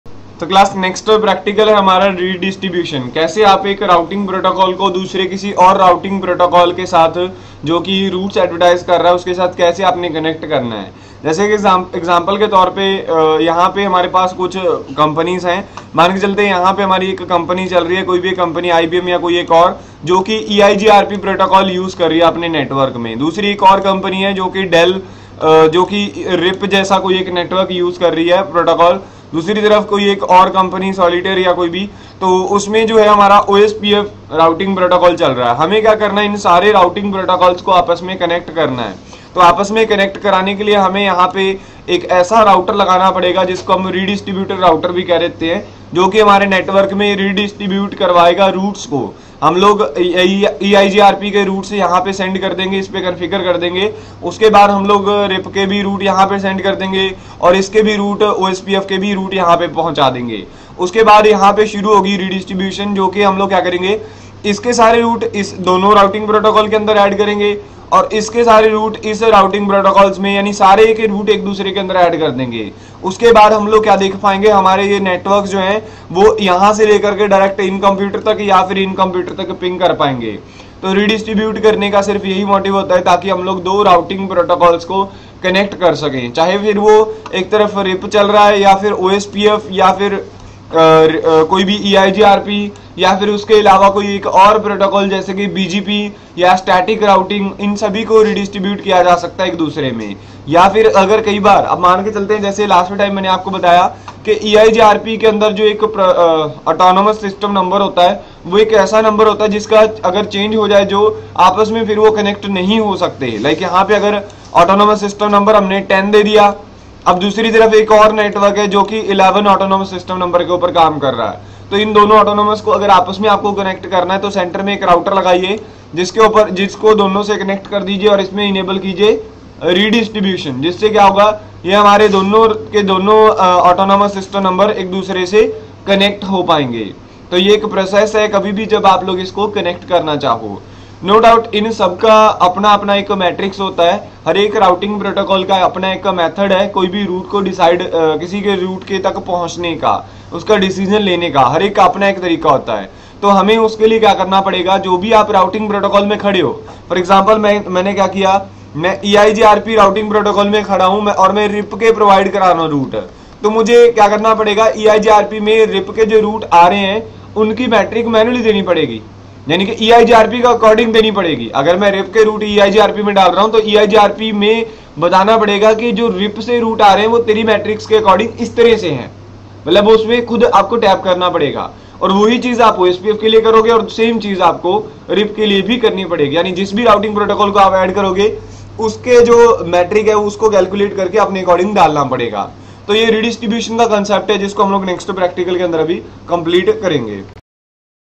Configuring RIP OSPF and EIGRP Redistribution in Cisco Packet Tracer part 1
RIP OSPF and EIGRP Redistribution is a very impotent part of Routing.with the help of  Redistribution we can create a connection between two or more routing protocols like RIP OSPF and EIGRP